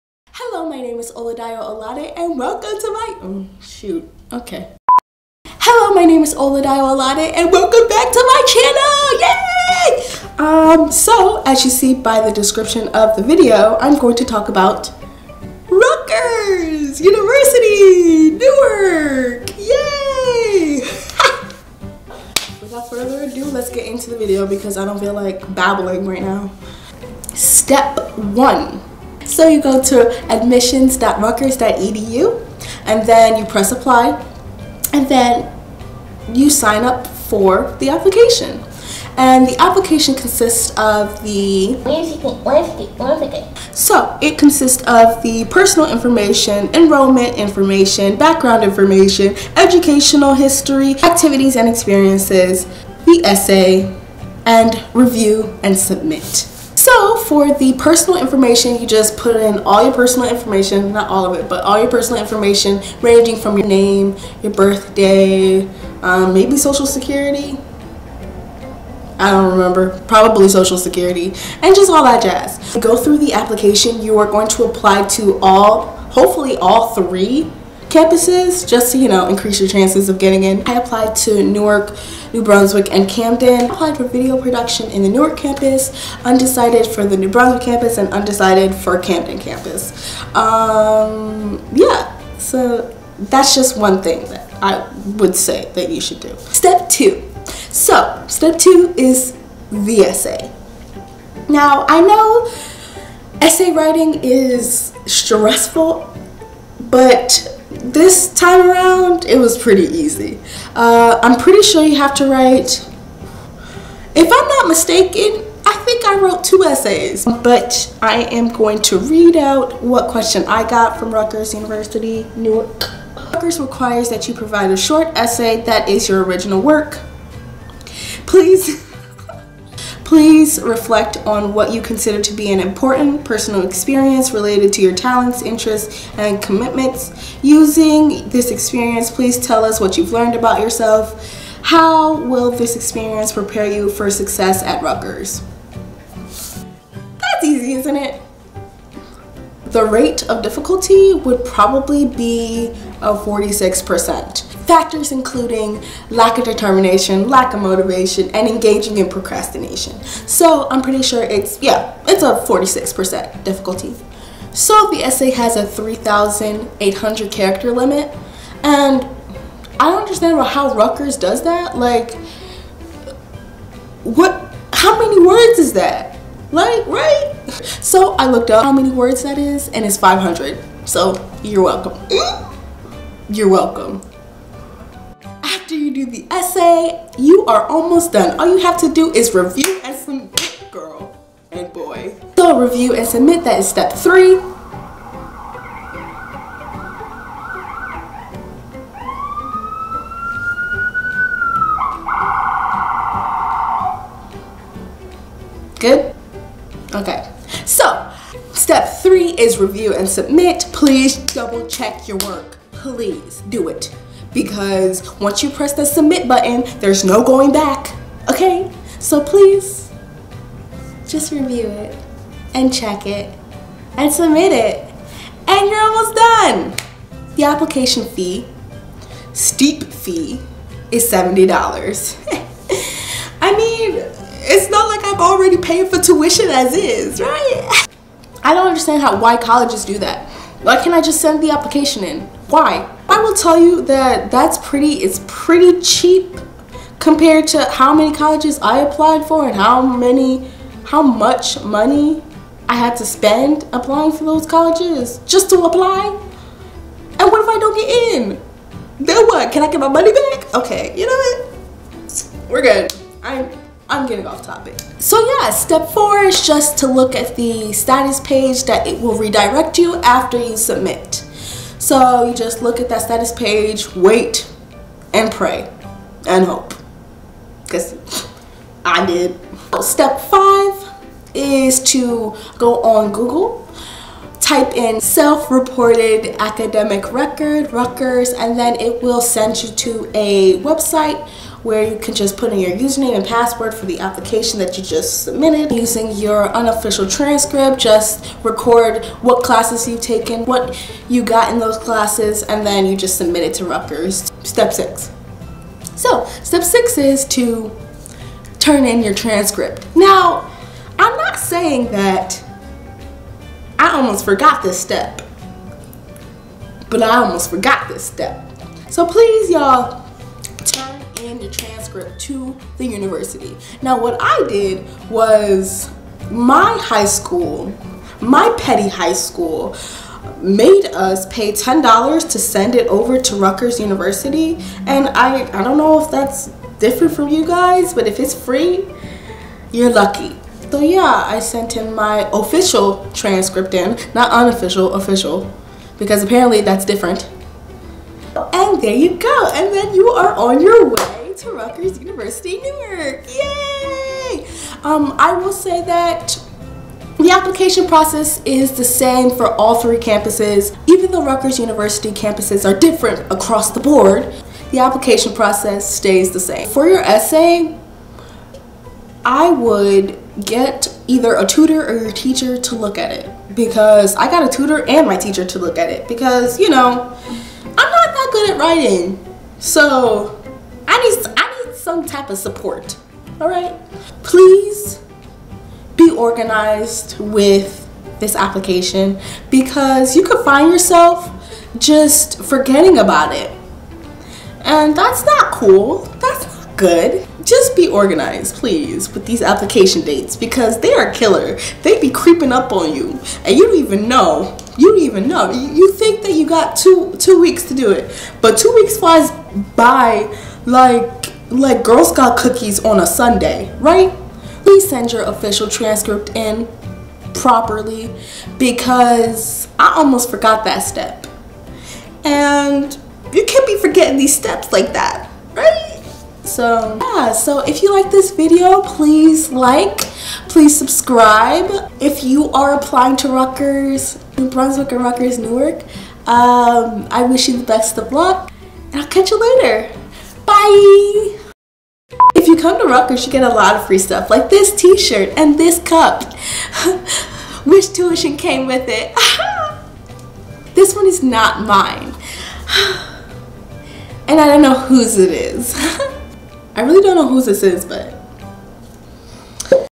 ALL ABOUT APPLYING TO RUTGERS UNIVERSITY-NEWARK
How to apply to Rutgers University Newark. Step by Step! Will be posting more about my Rutgers- Newark experience, the good and the bad.